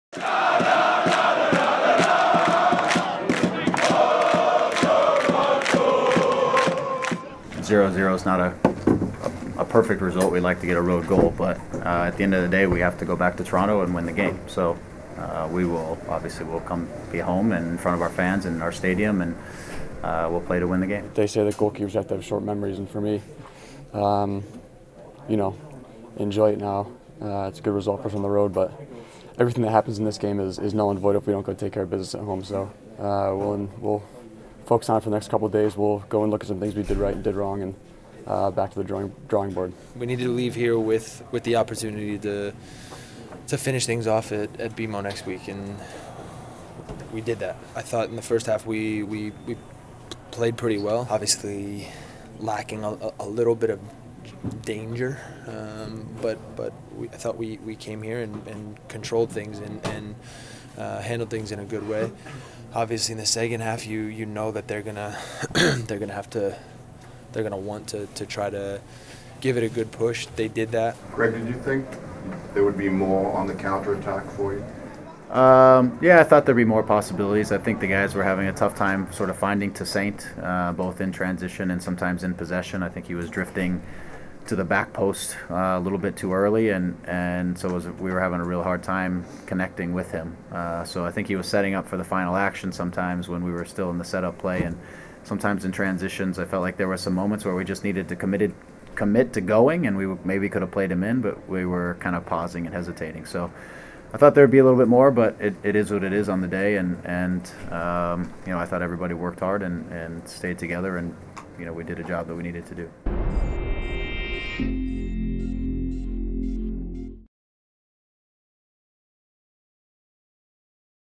Toronto FC banks on home game after tying Columbus 0-0
Toronto FC and Columbus Crew SC played to a 0-0 draw in the first leg of the MLS Eastern Conference final Tuesday. TFC goalkeeper Alex Bono says the game is “null and void” if the team loses the second leg in Toronto next week. (Nov. 22)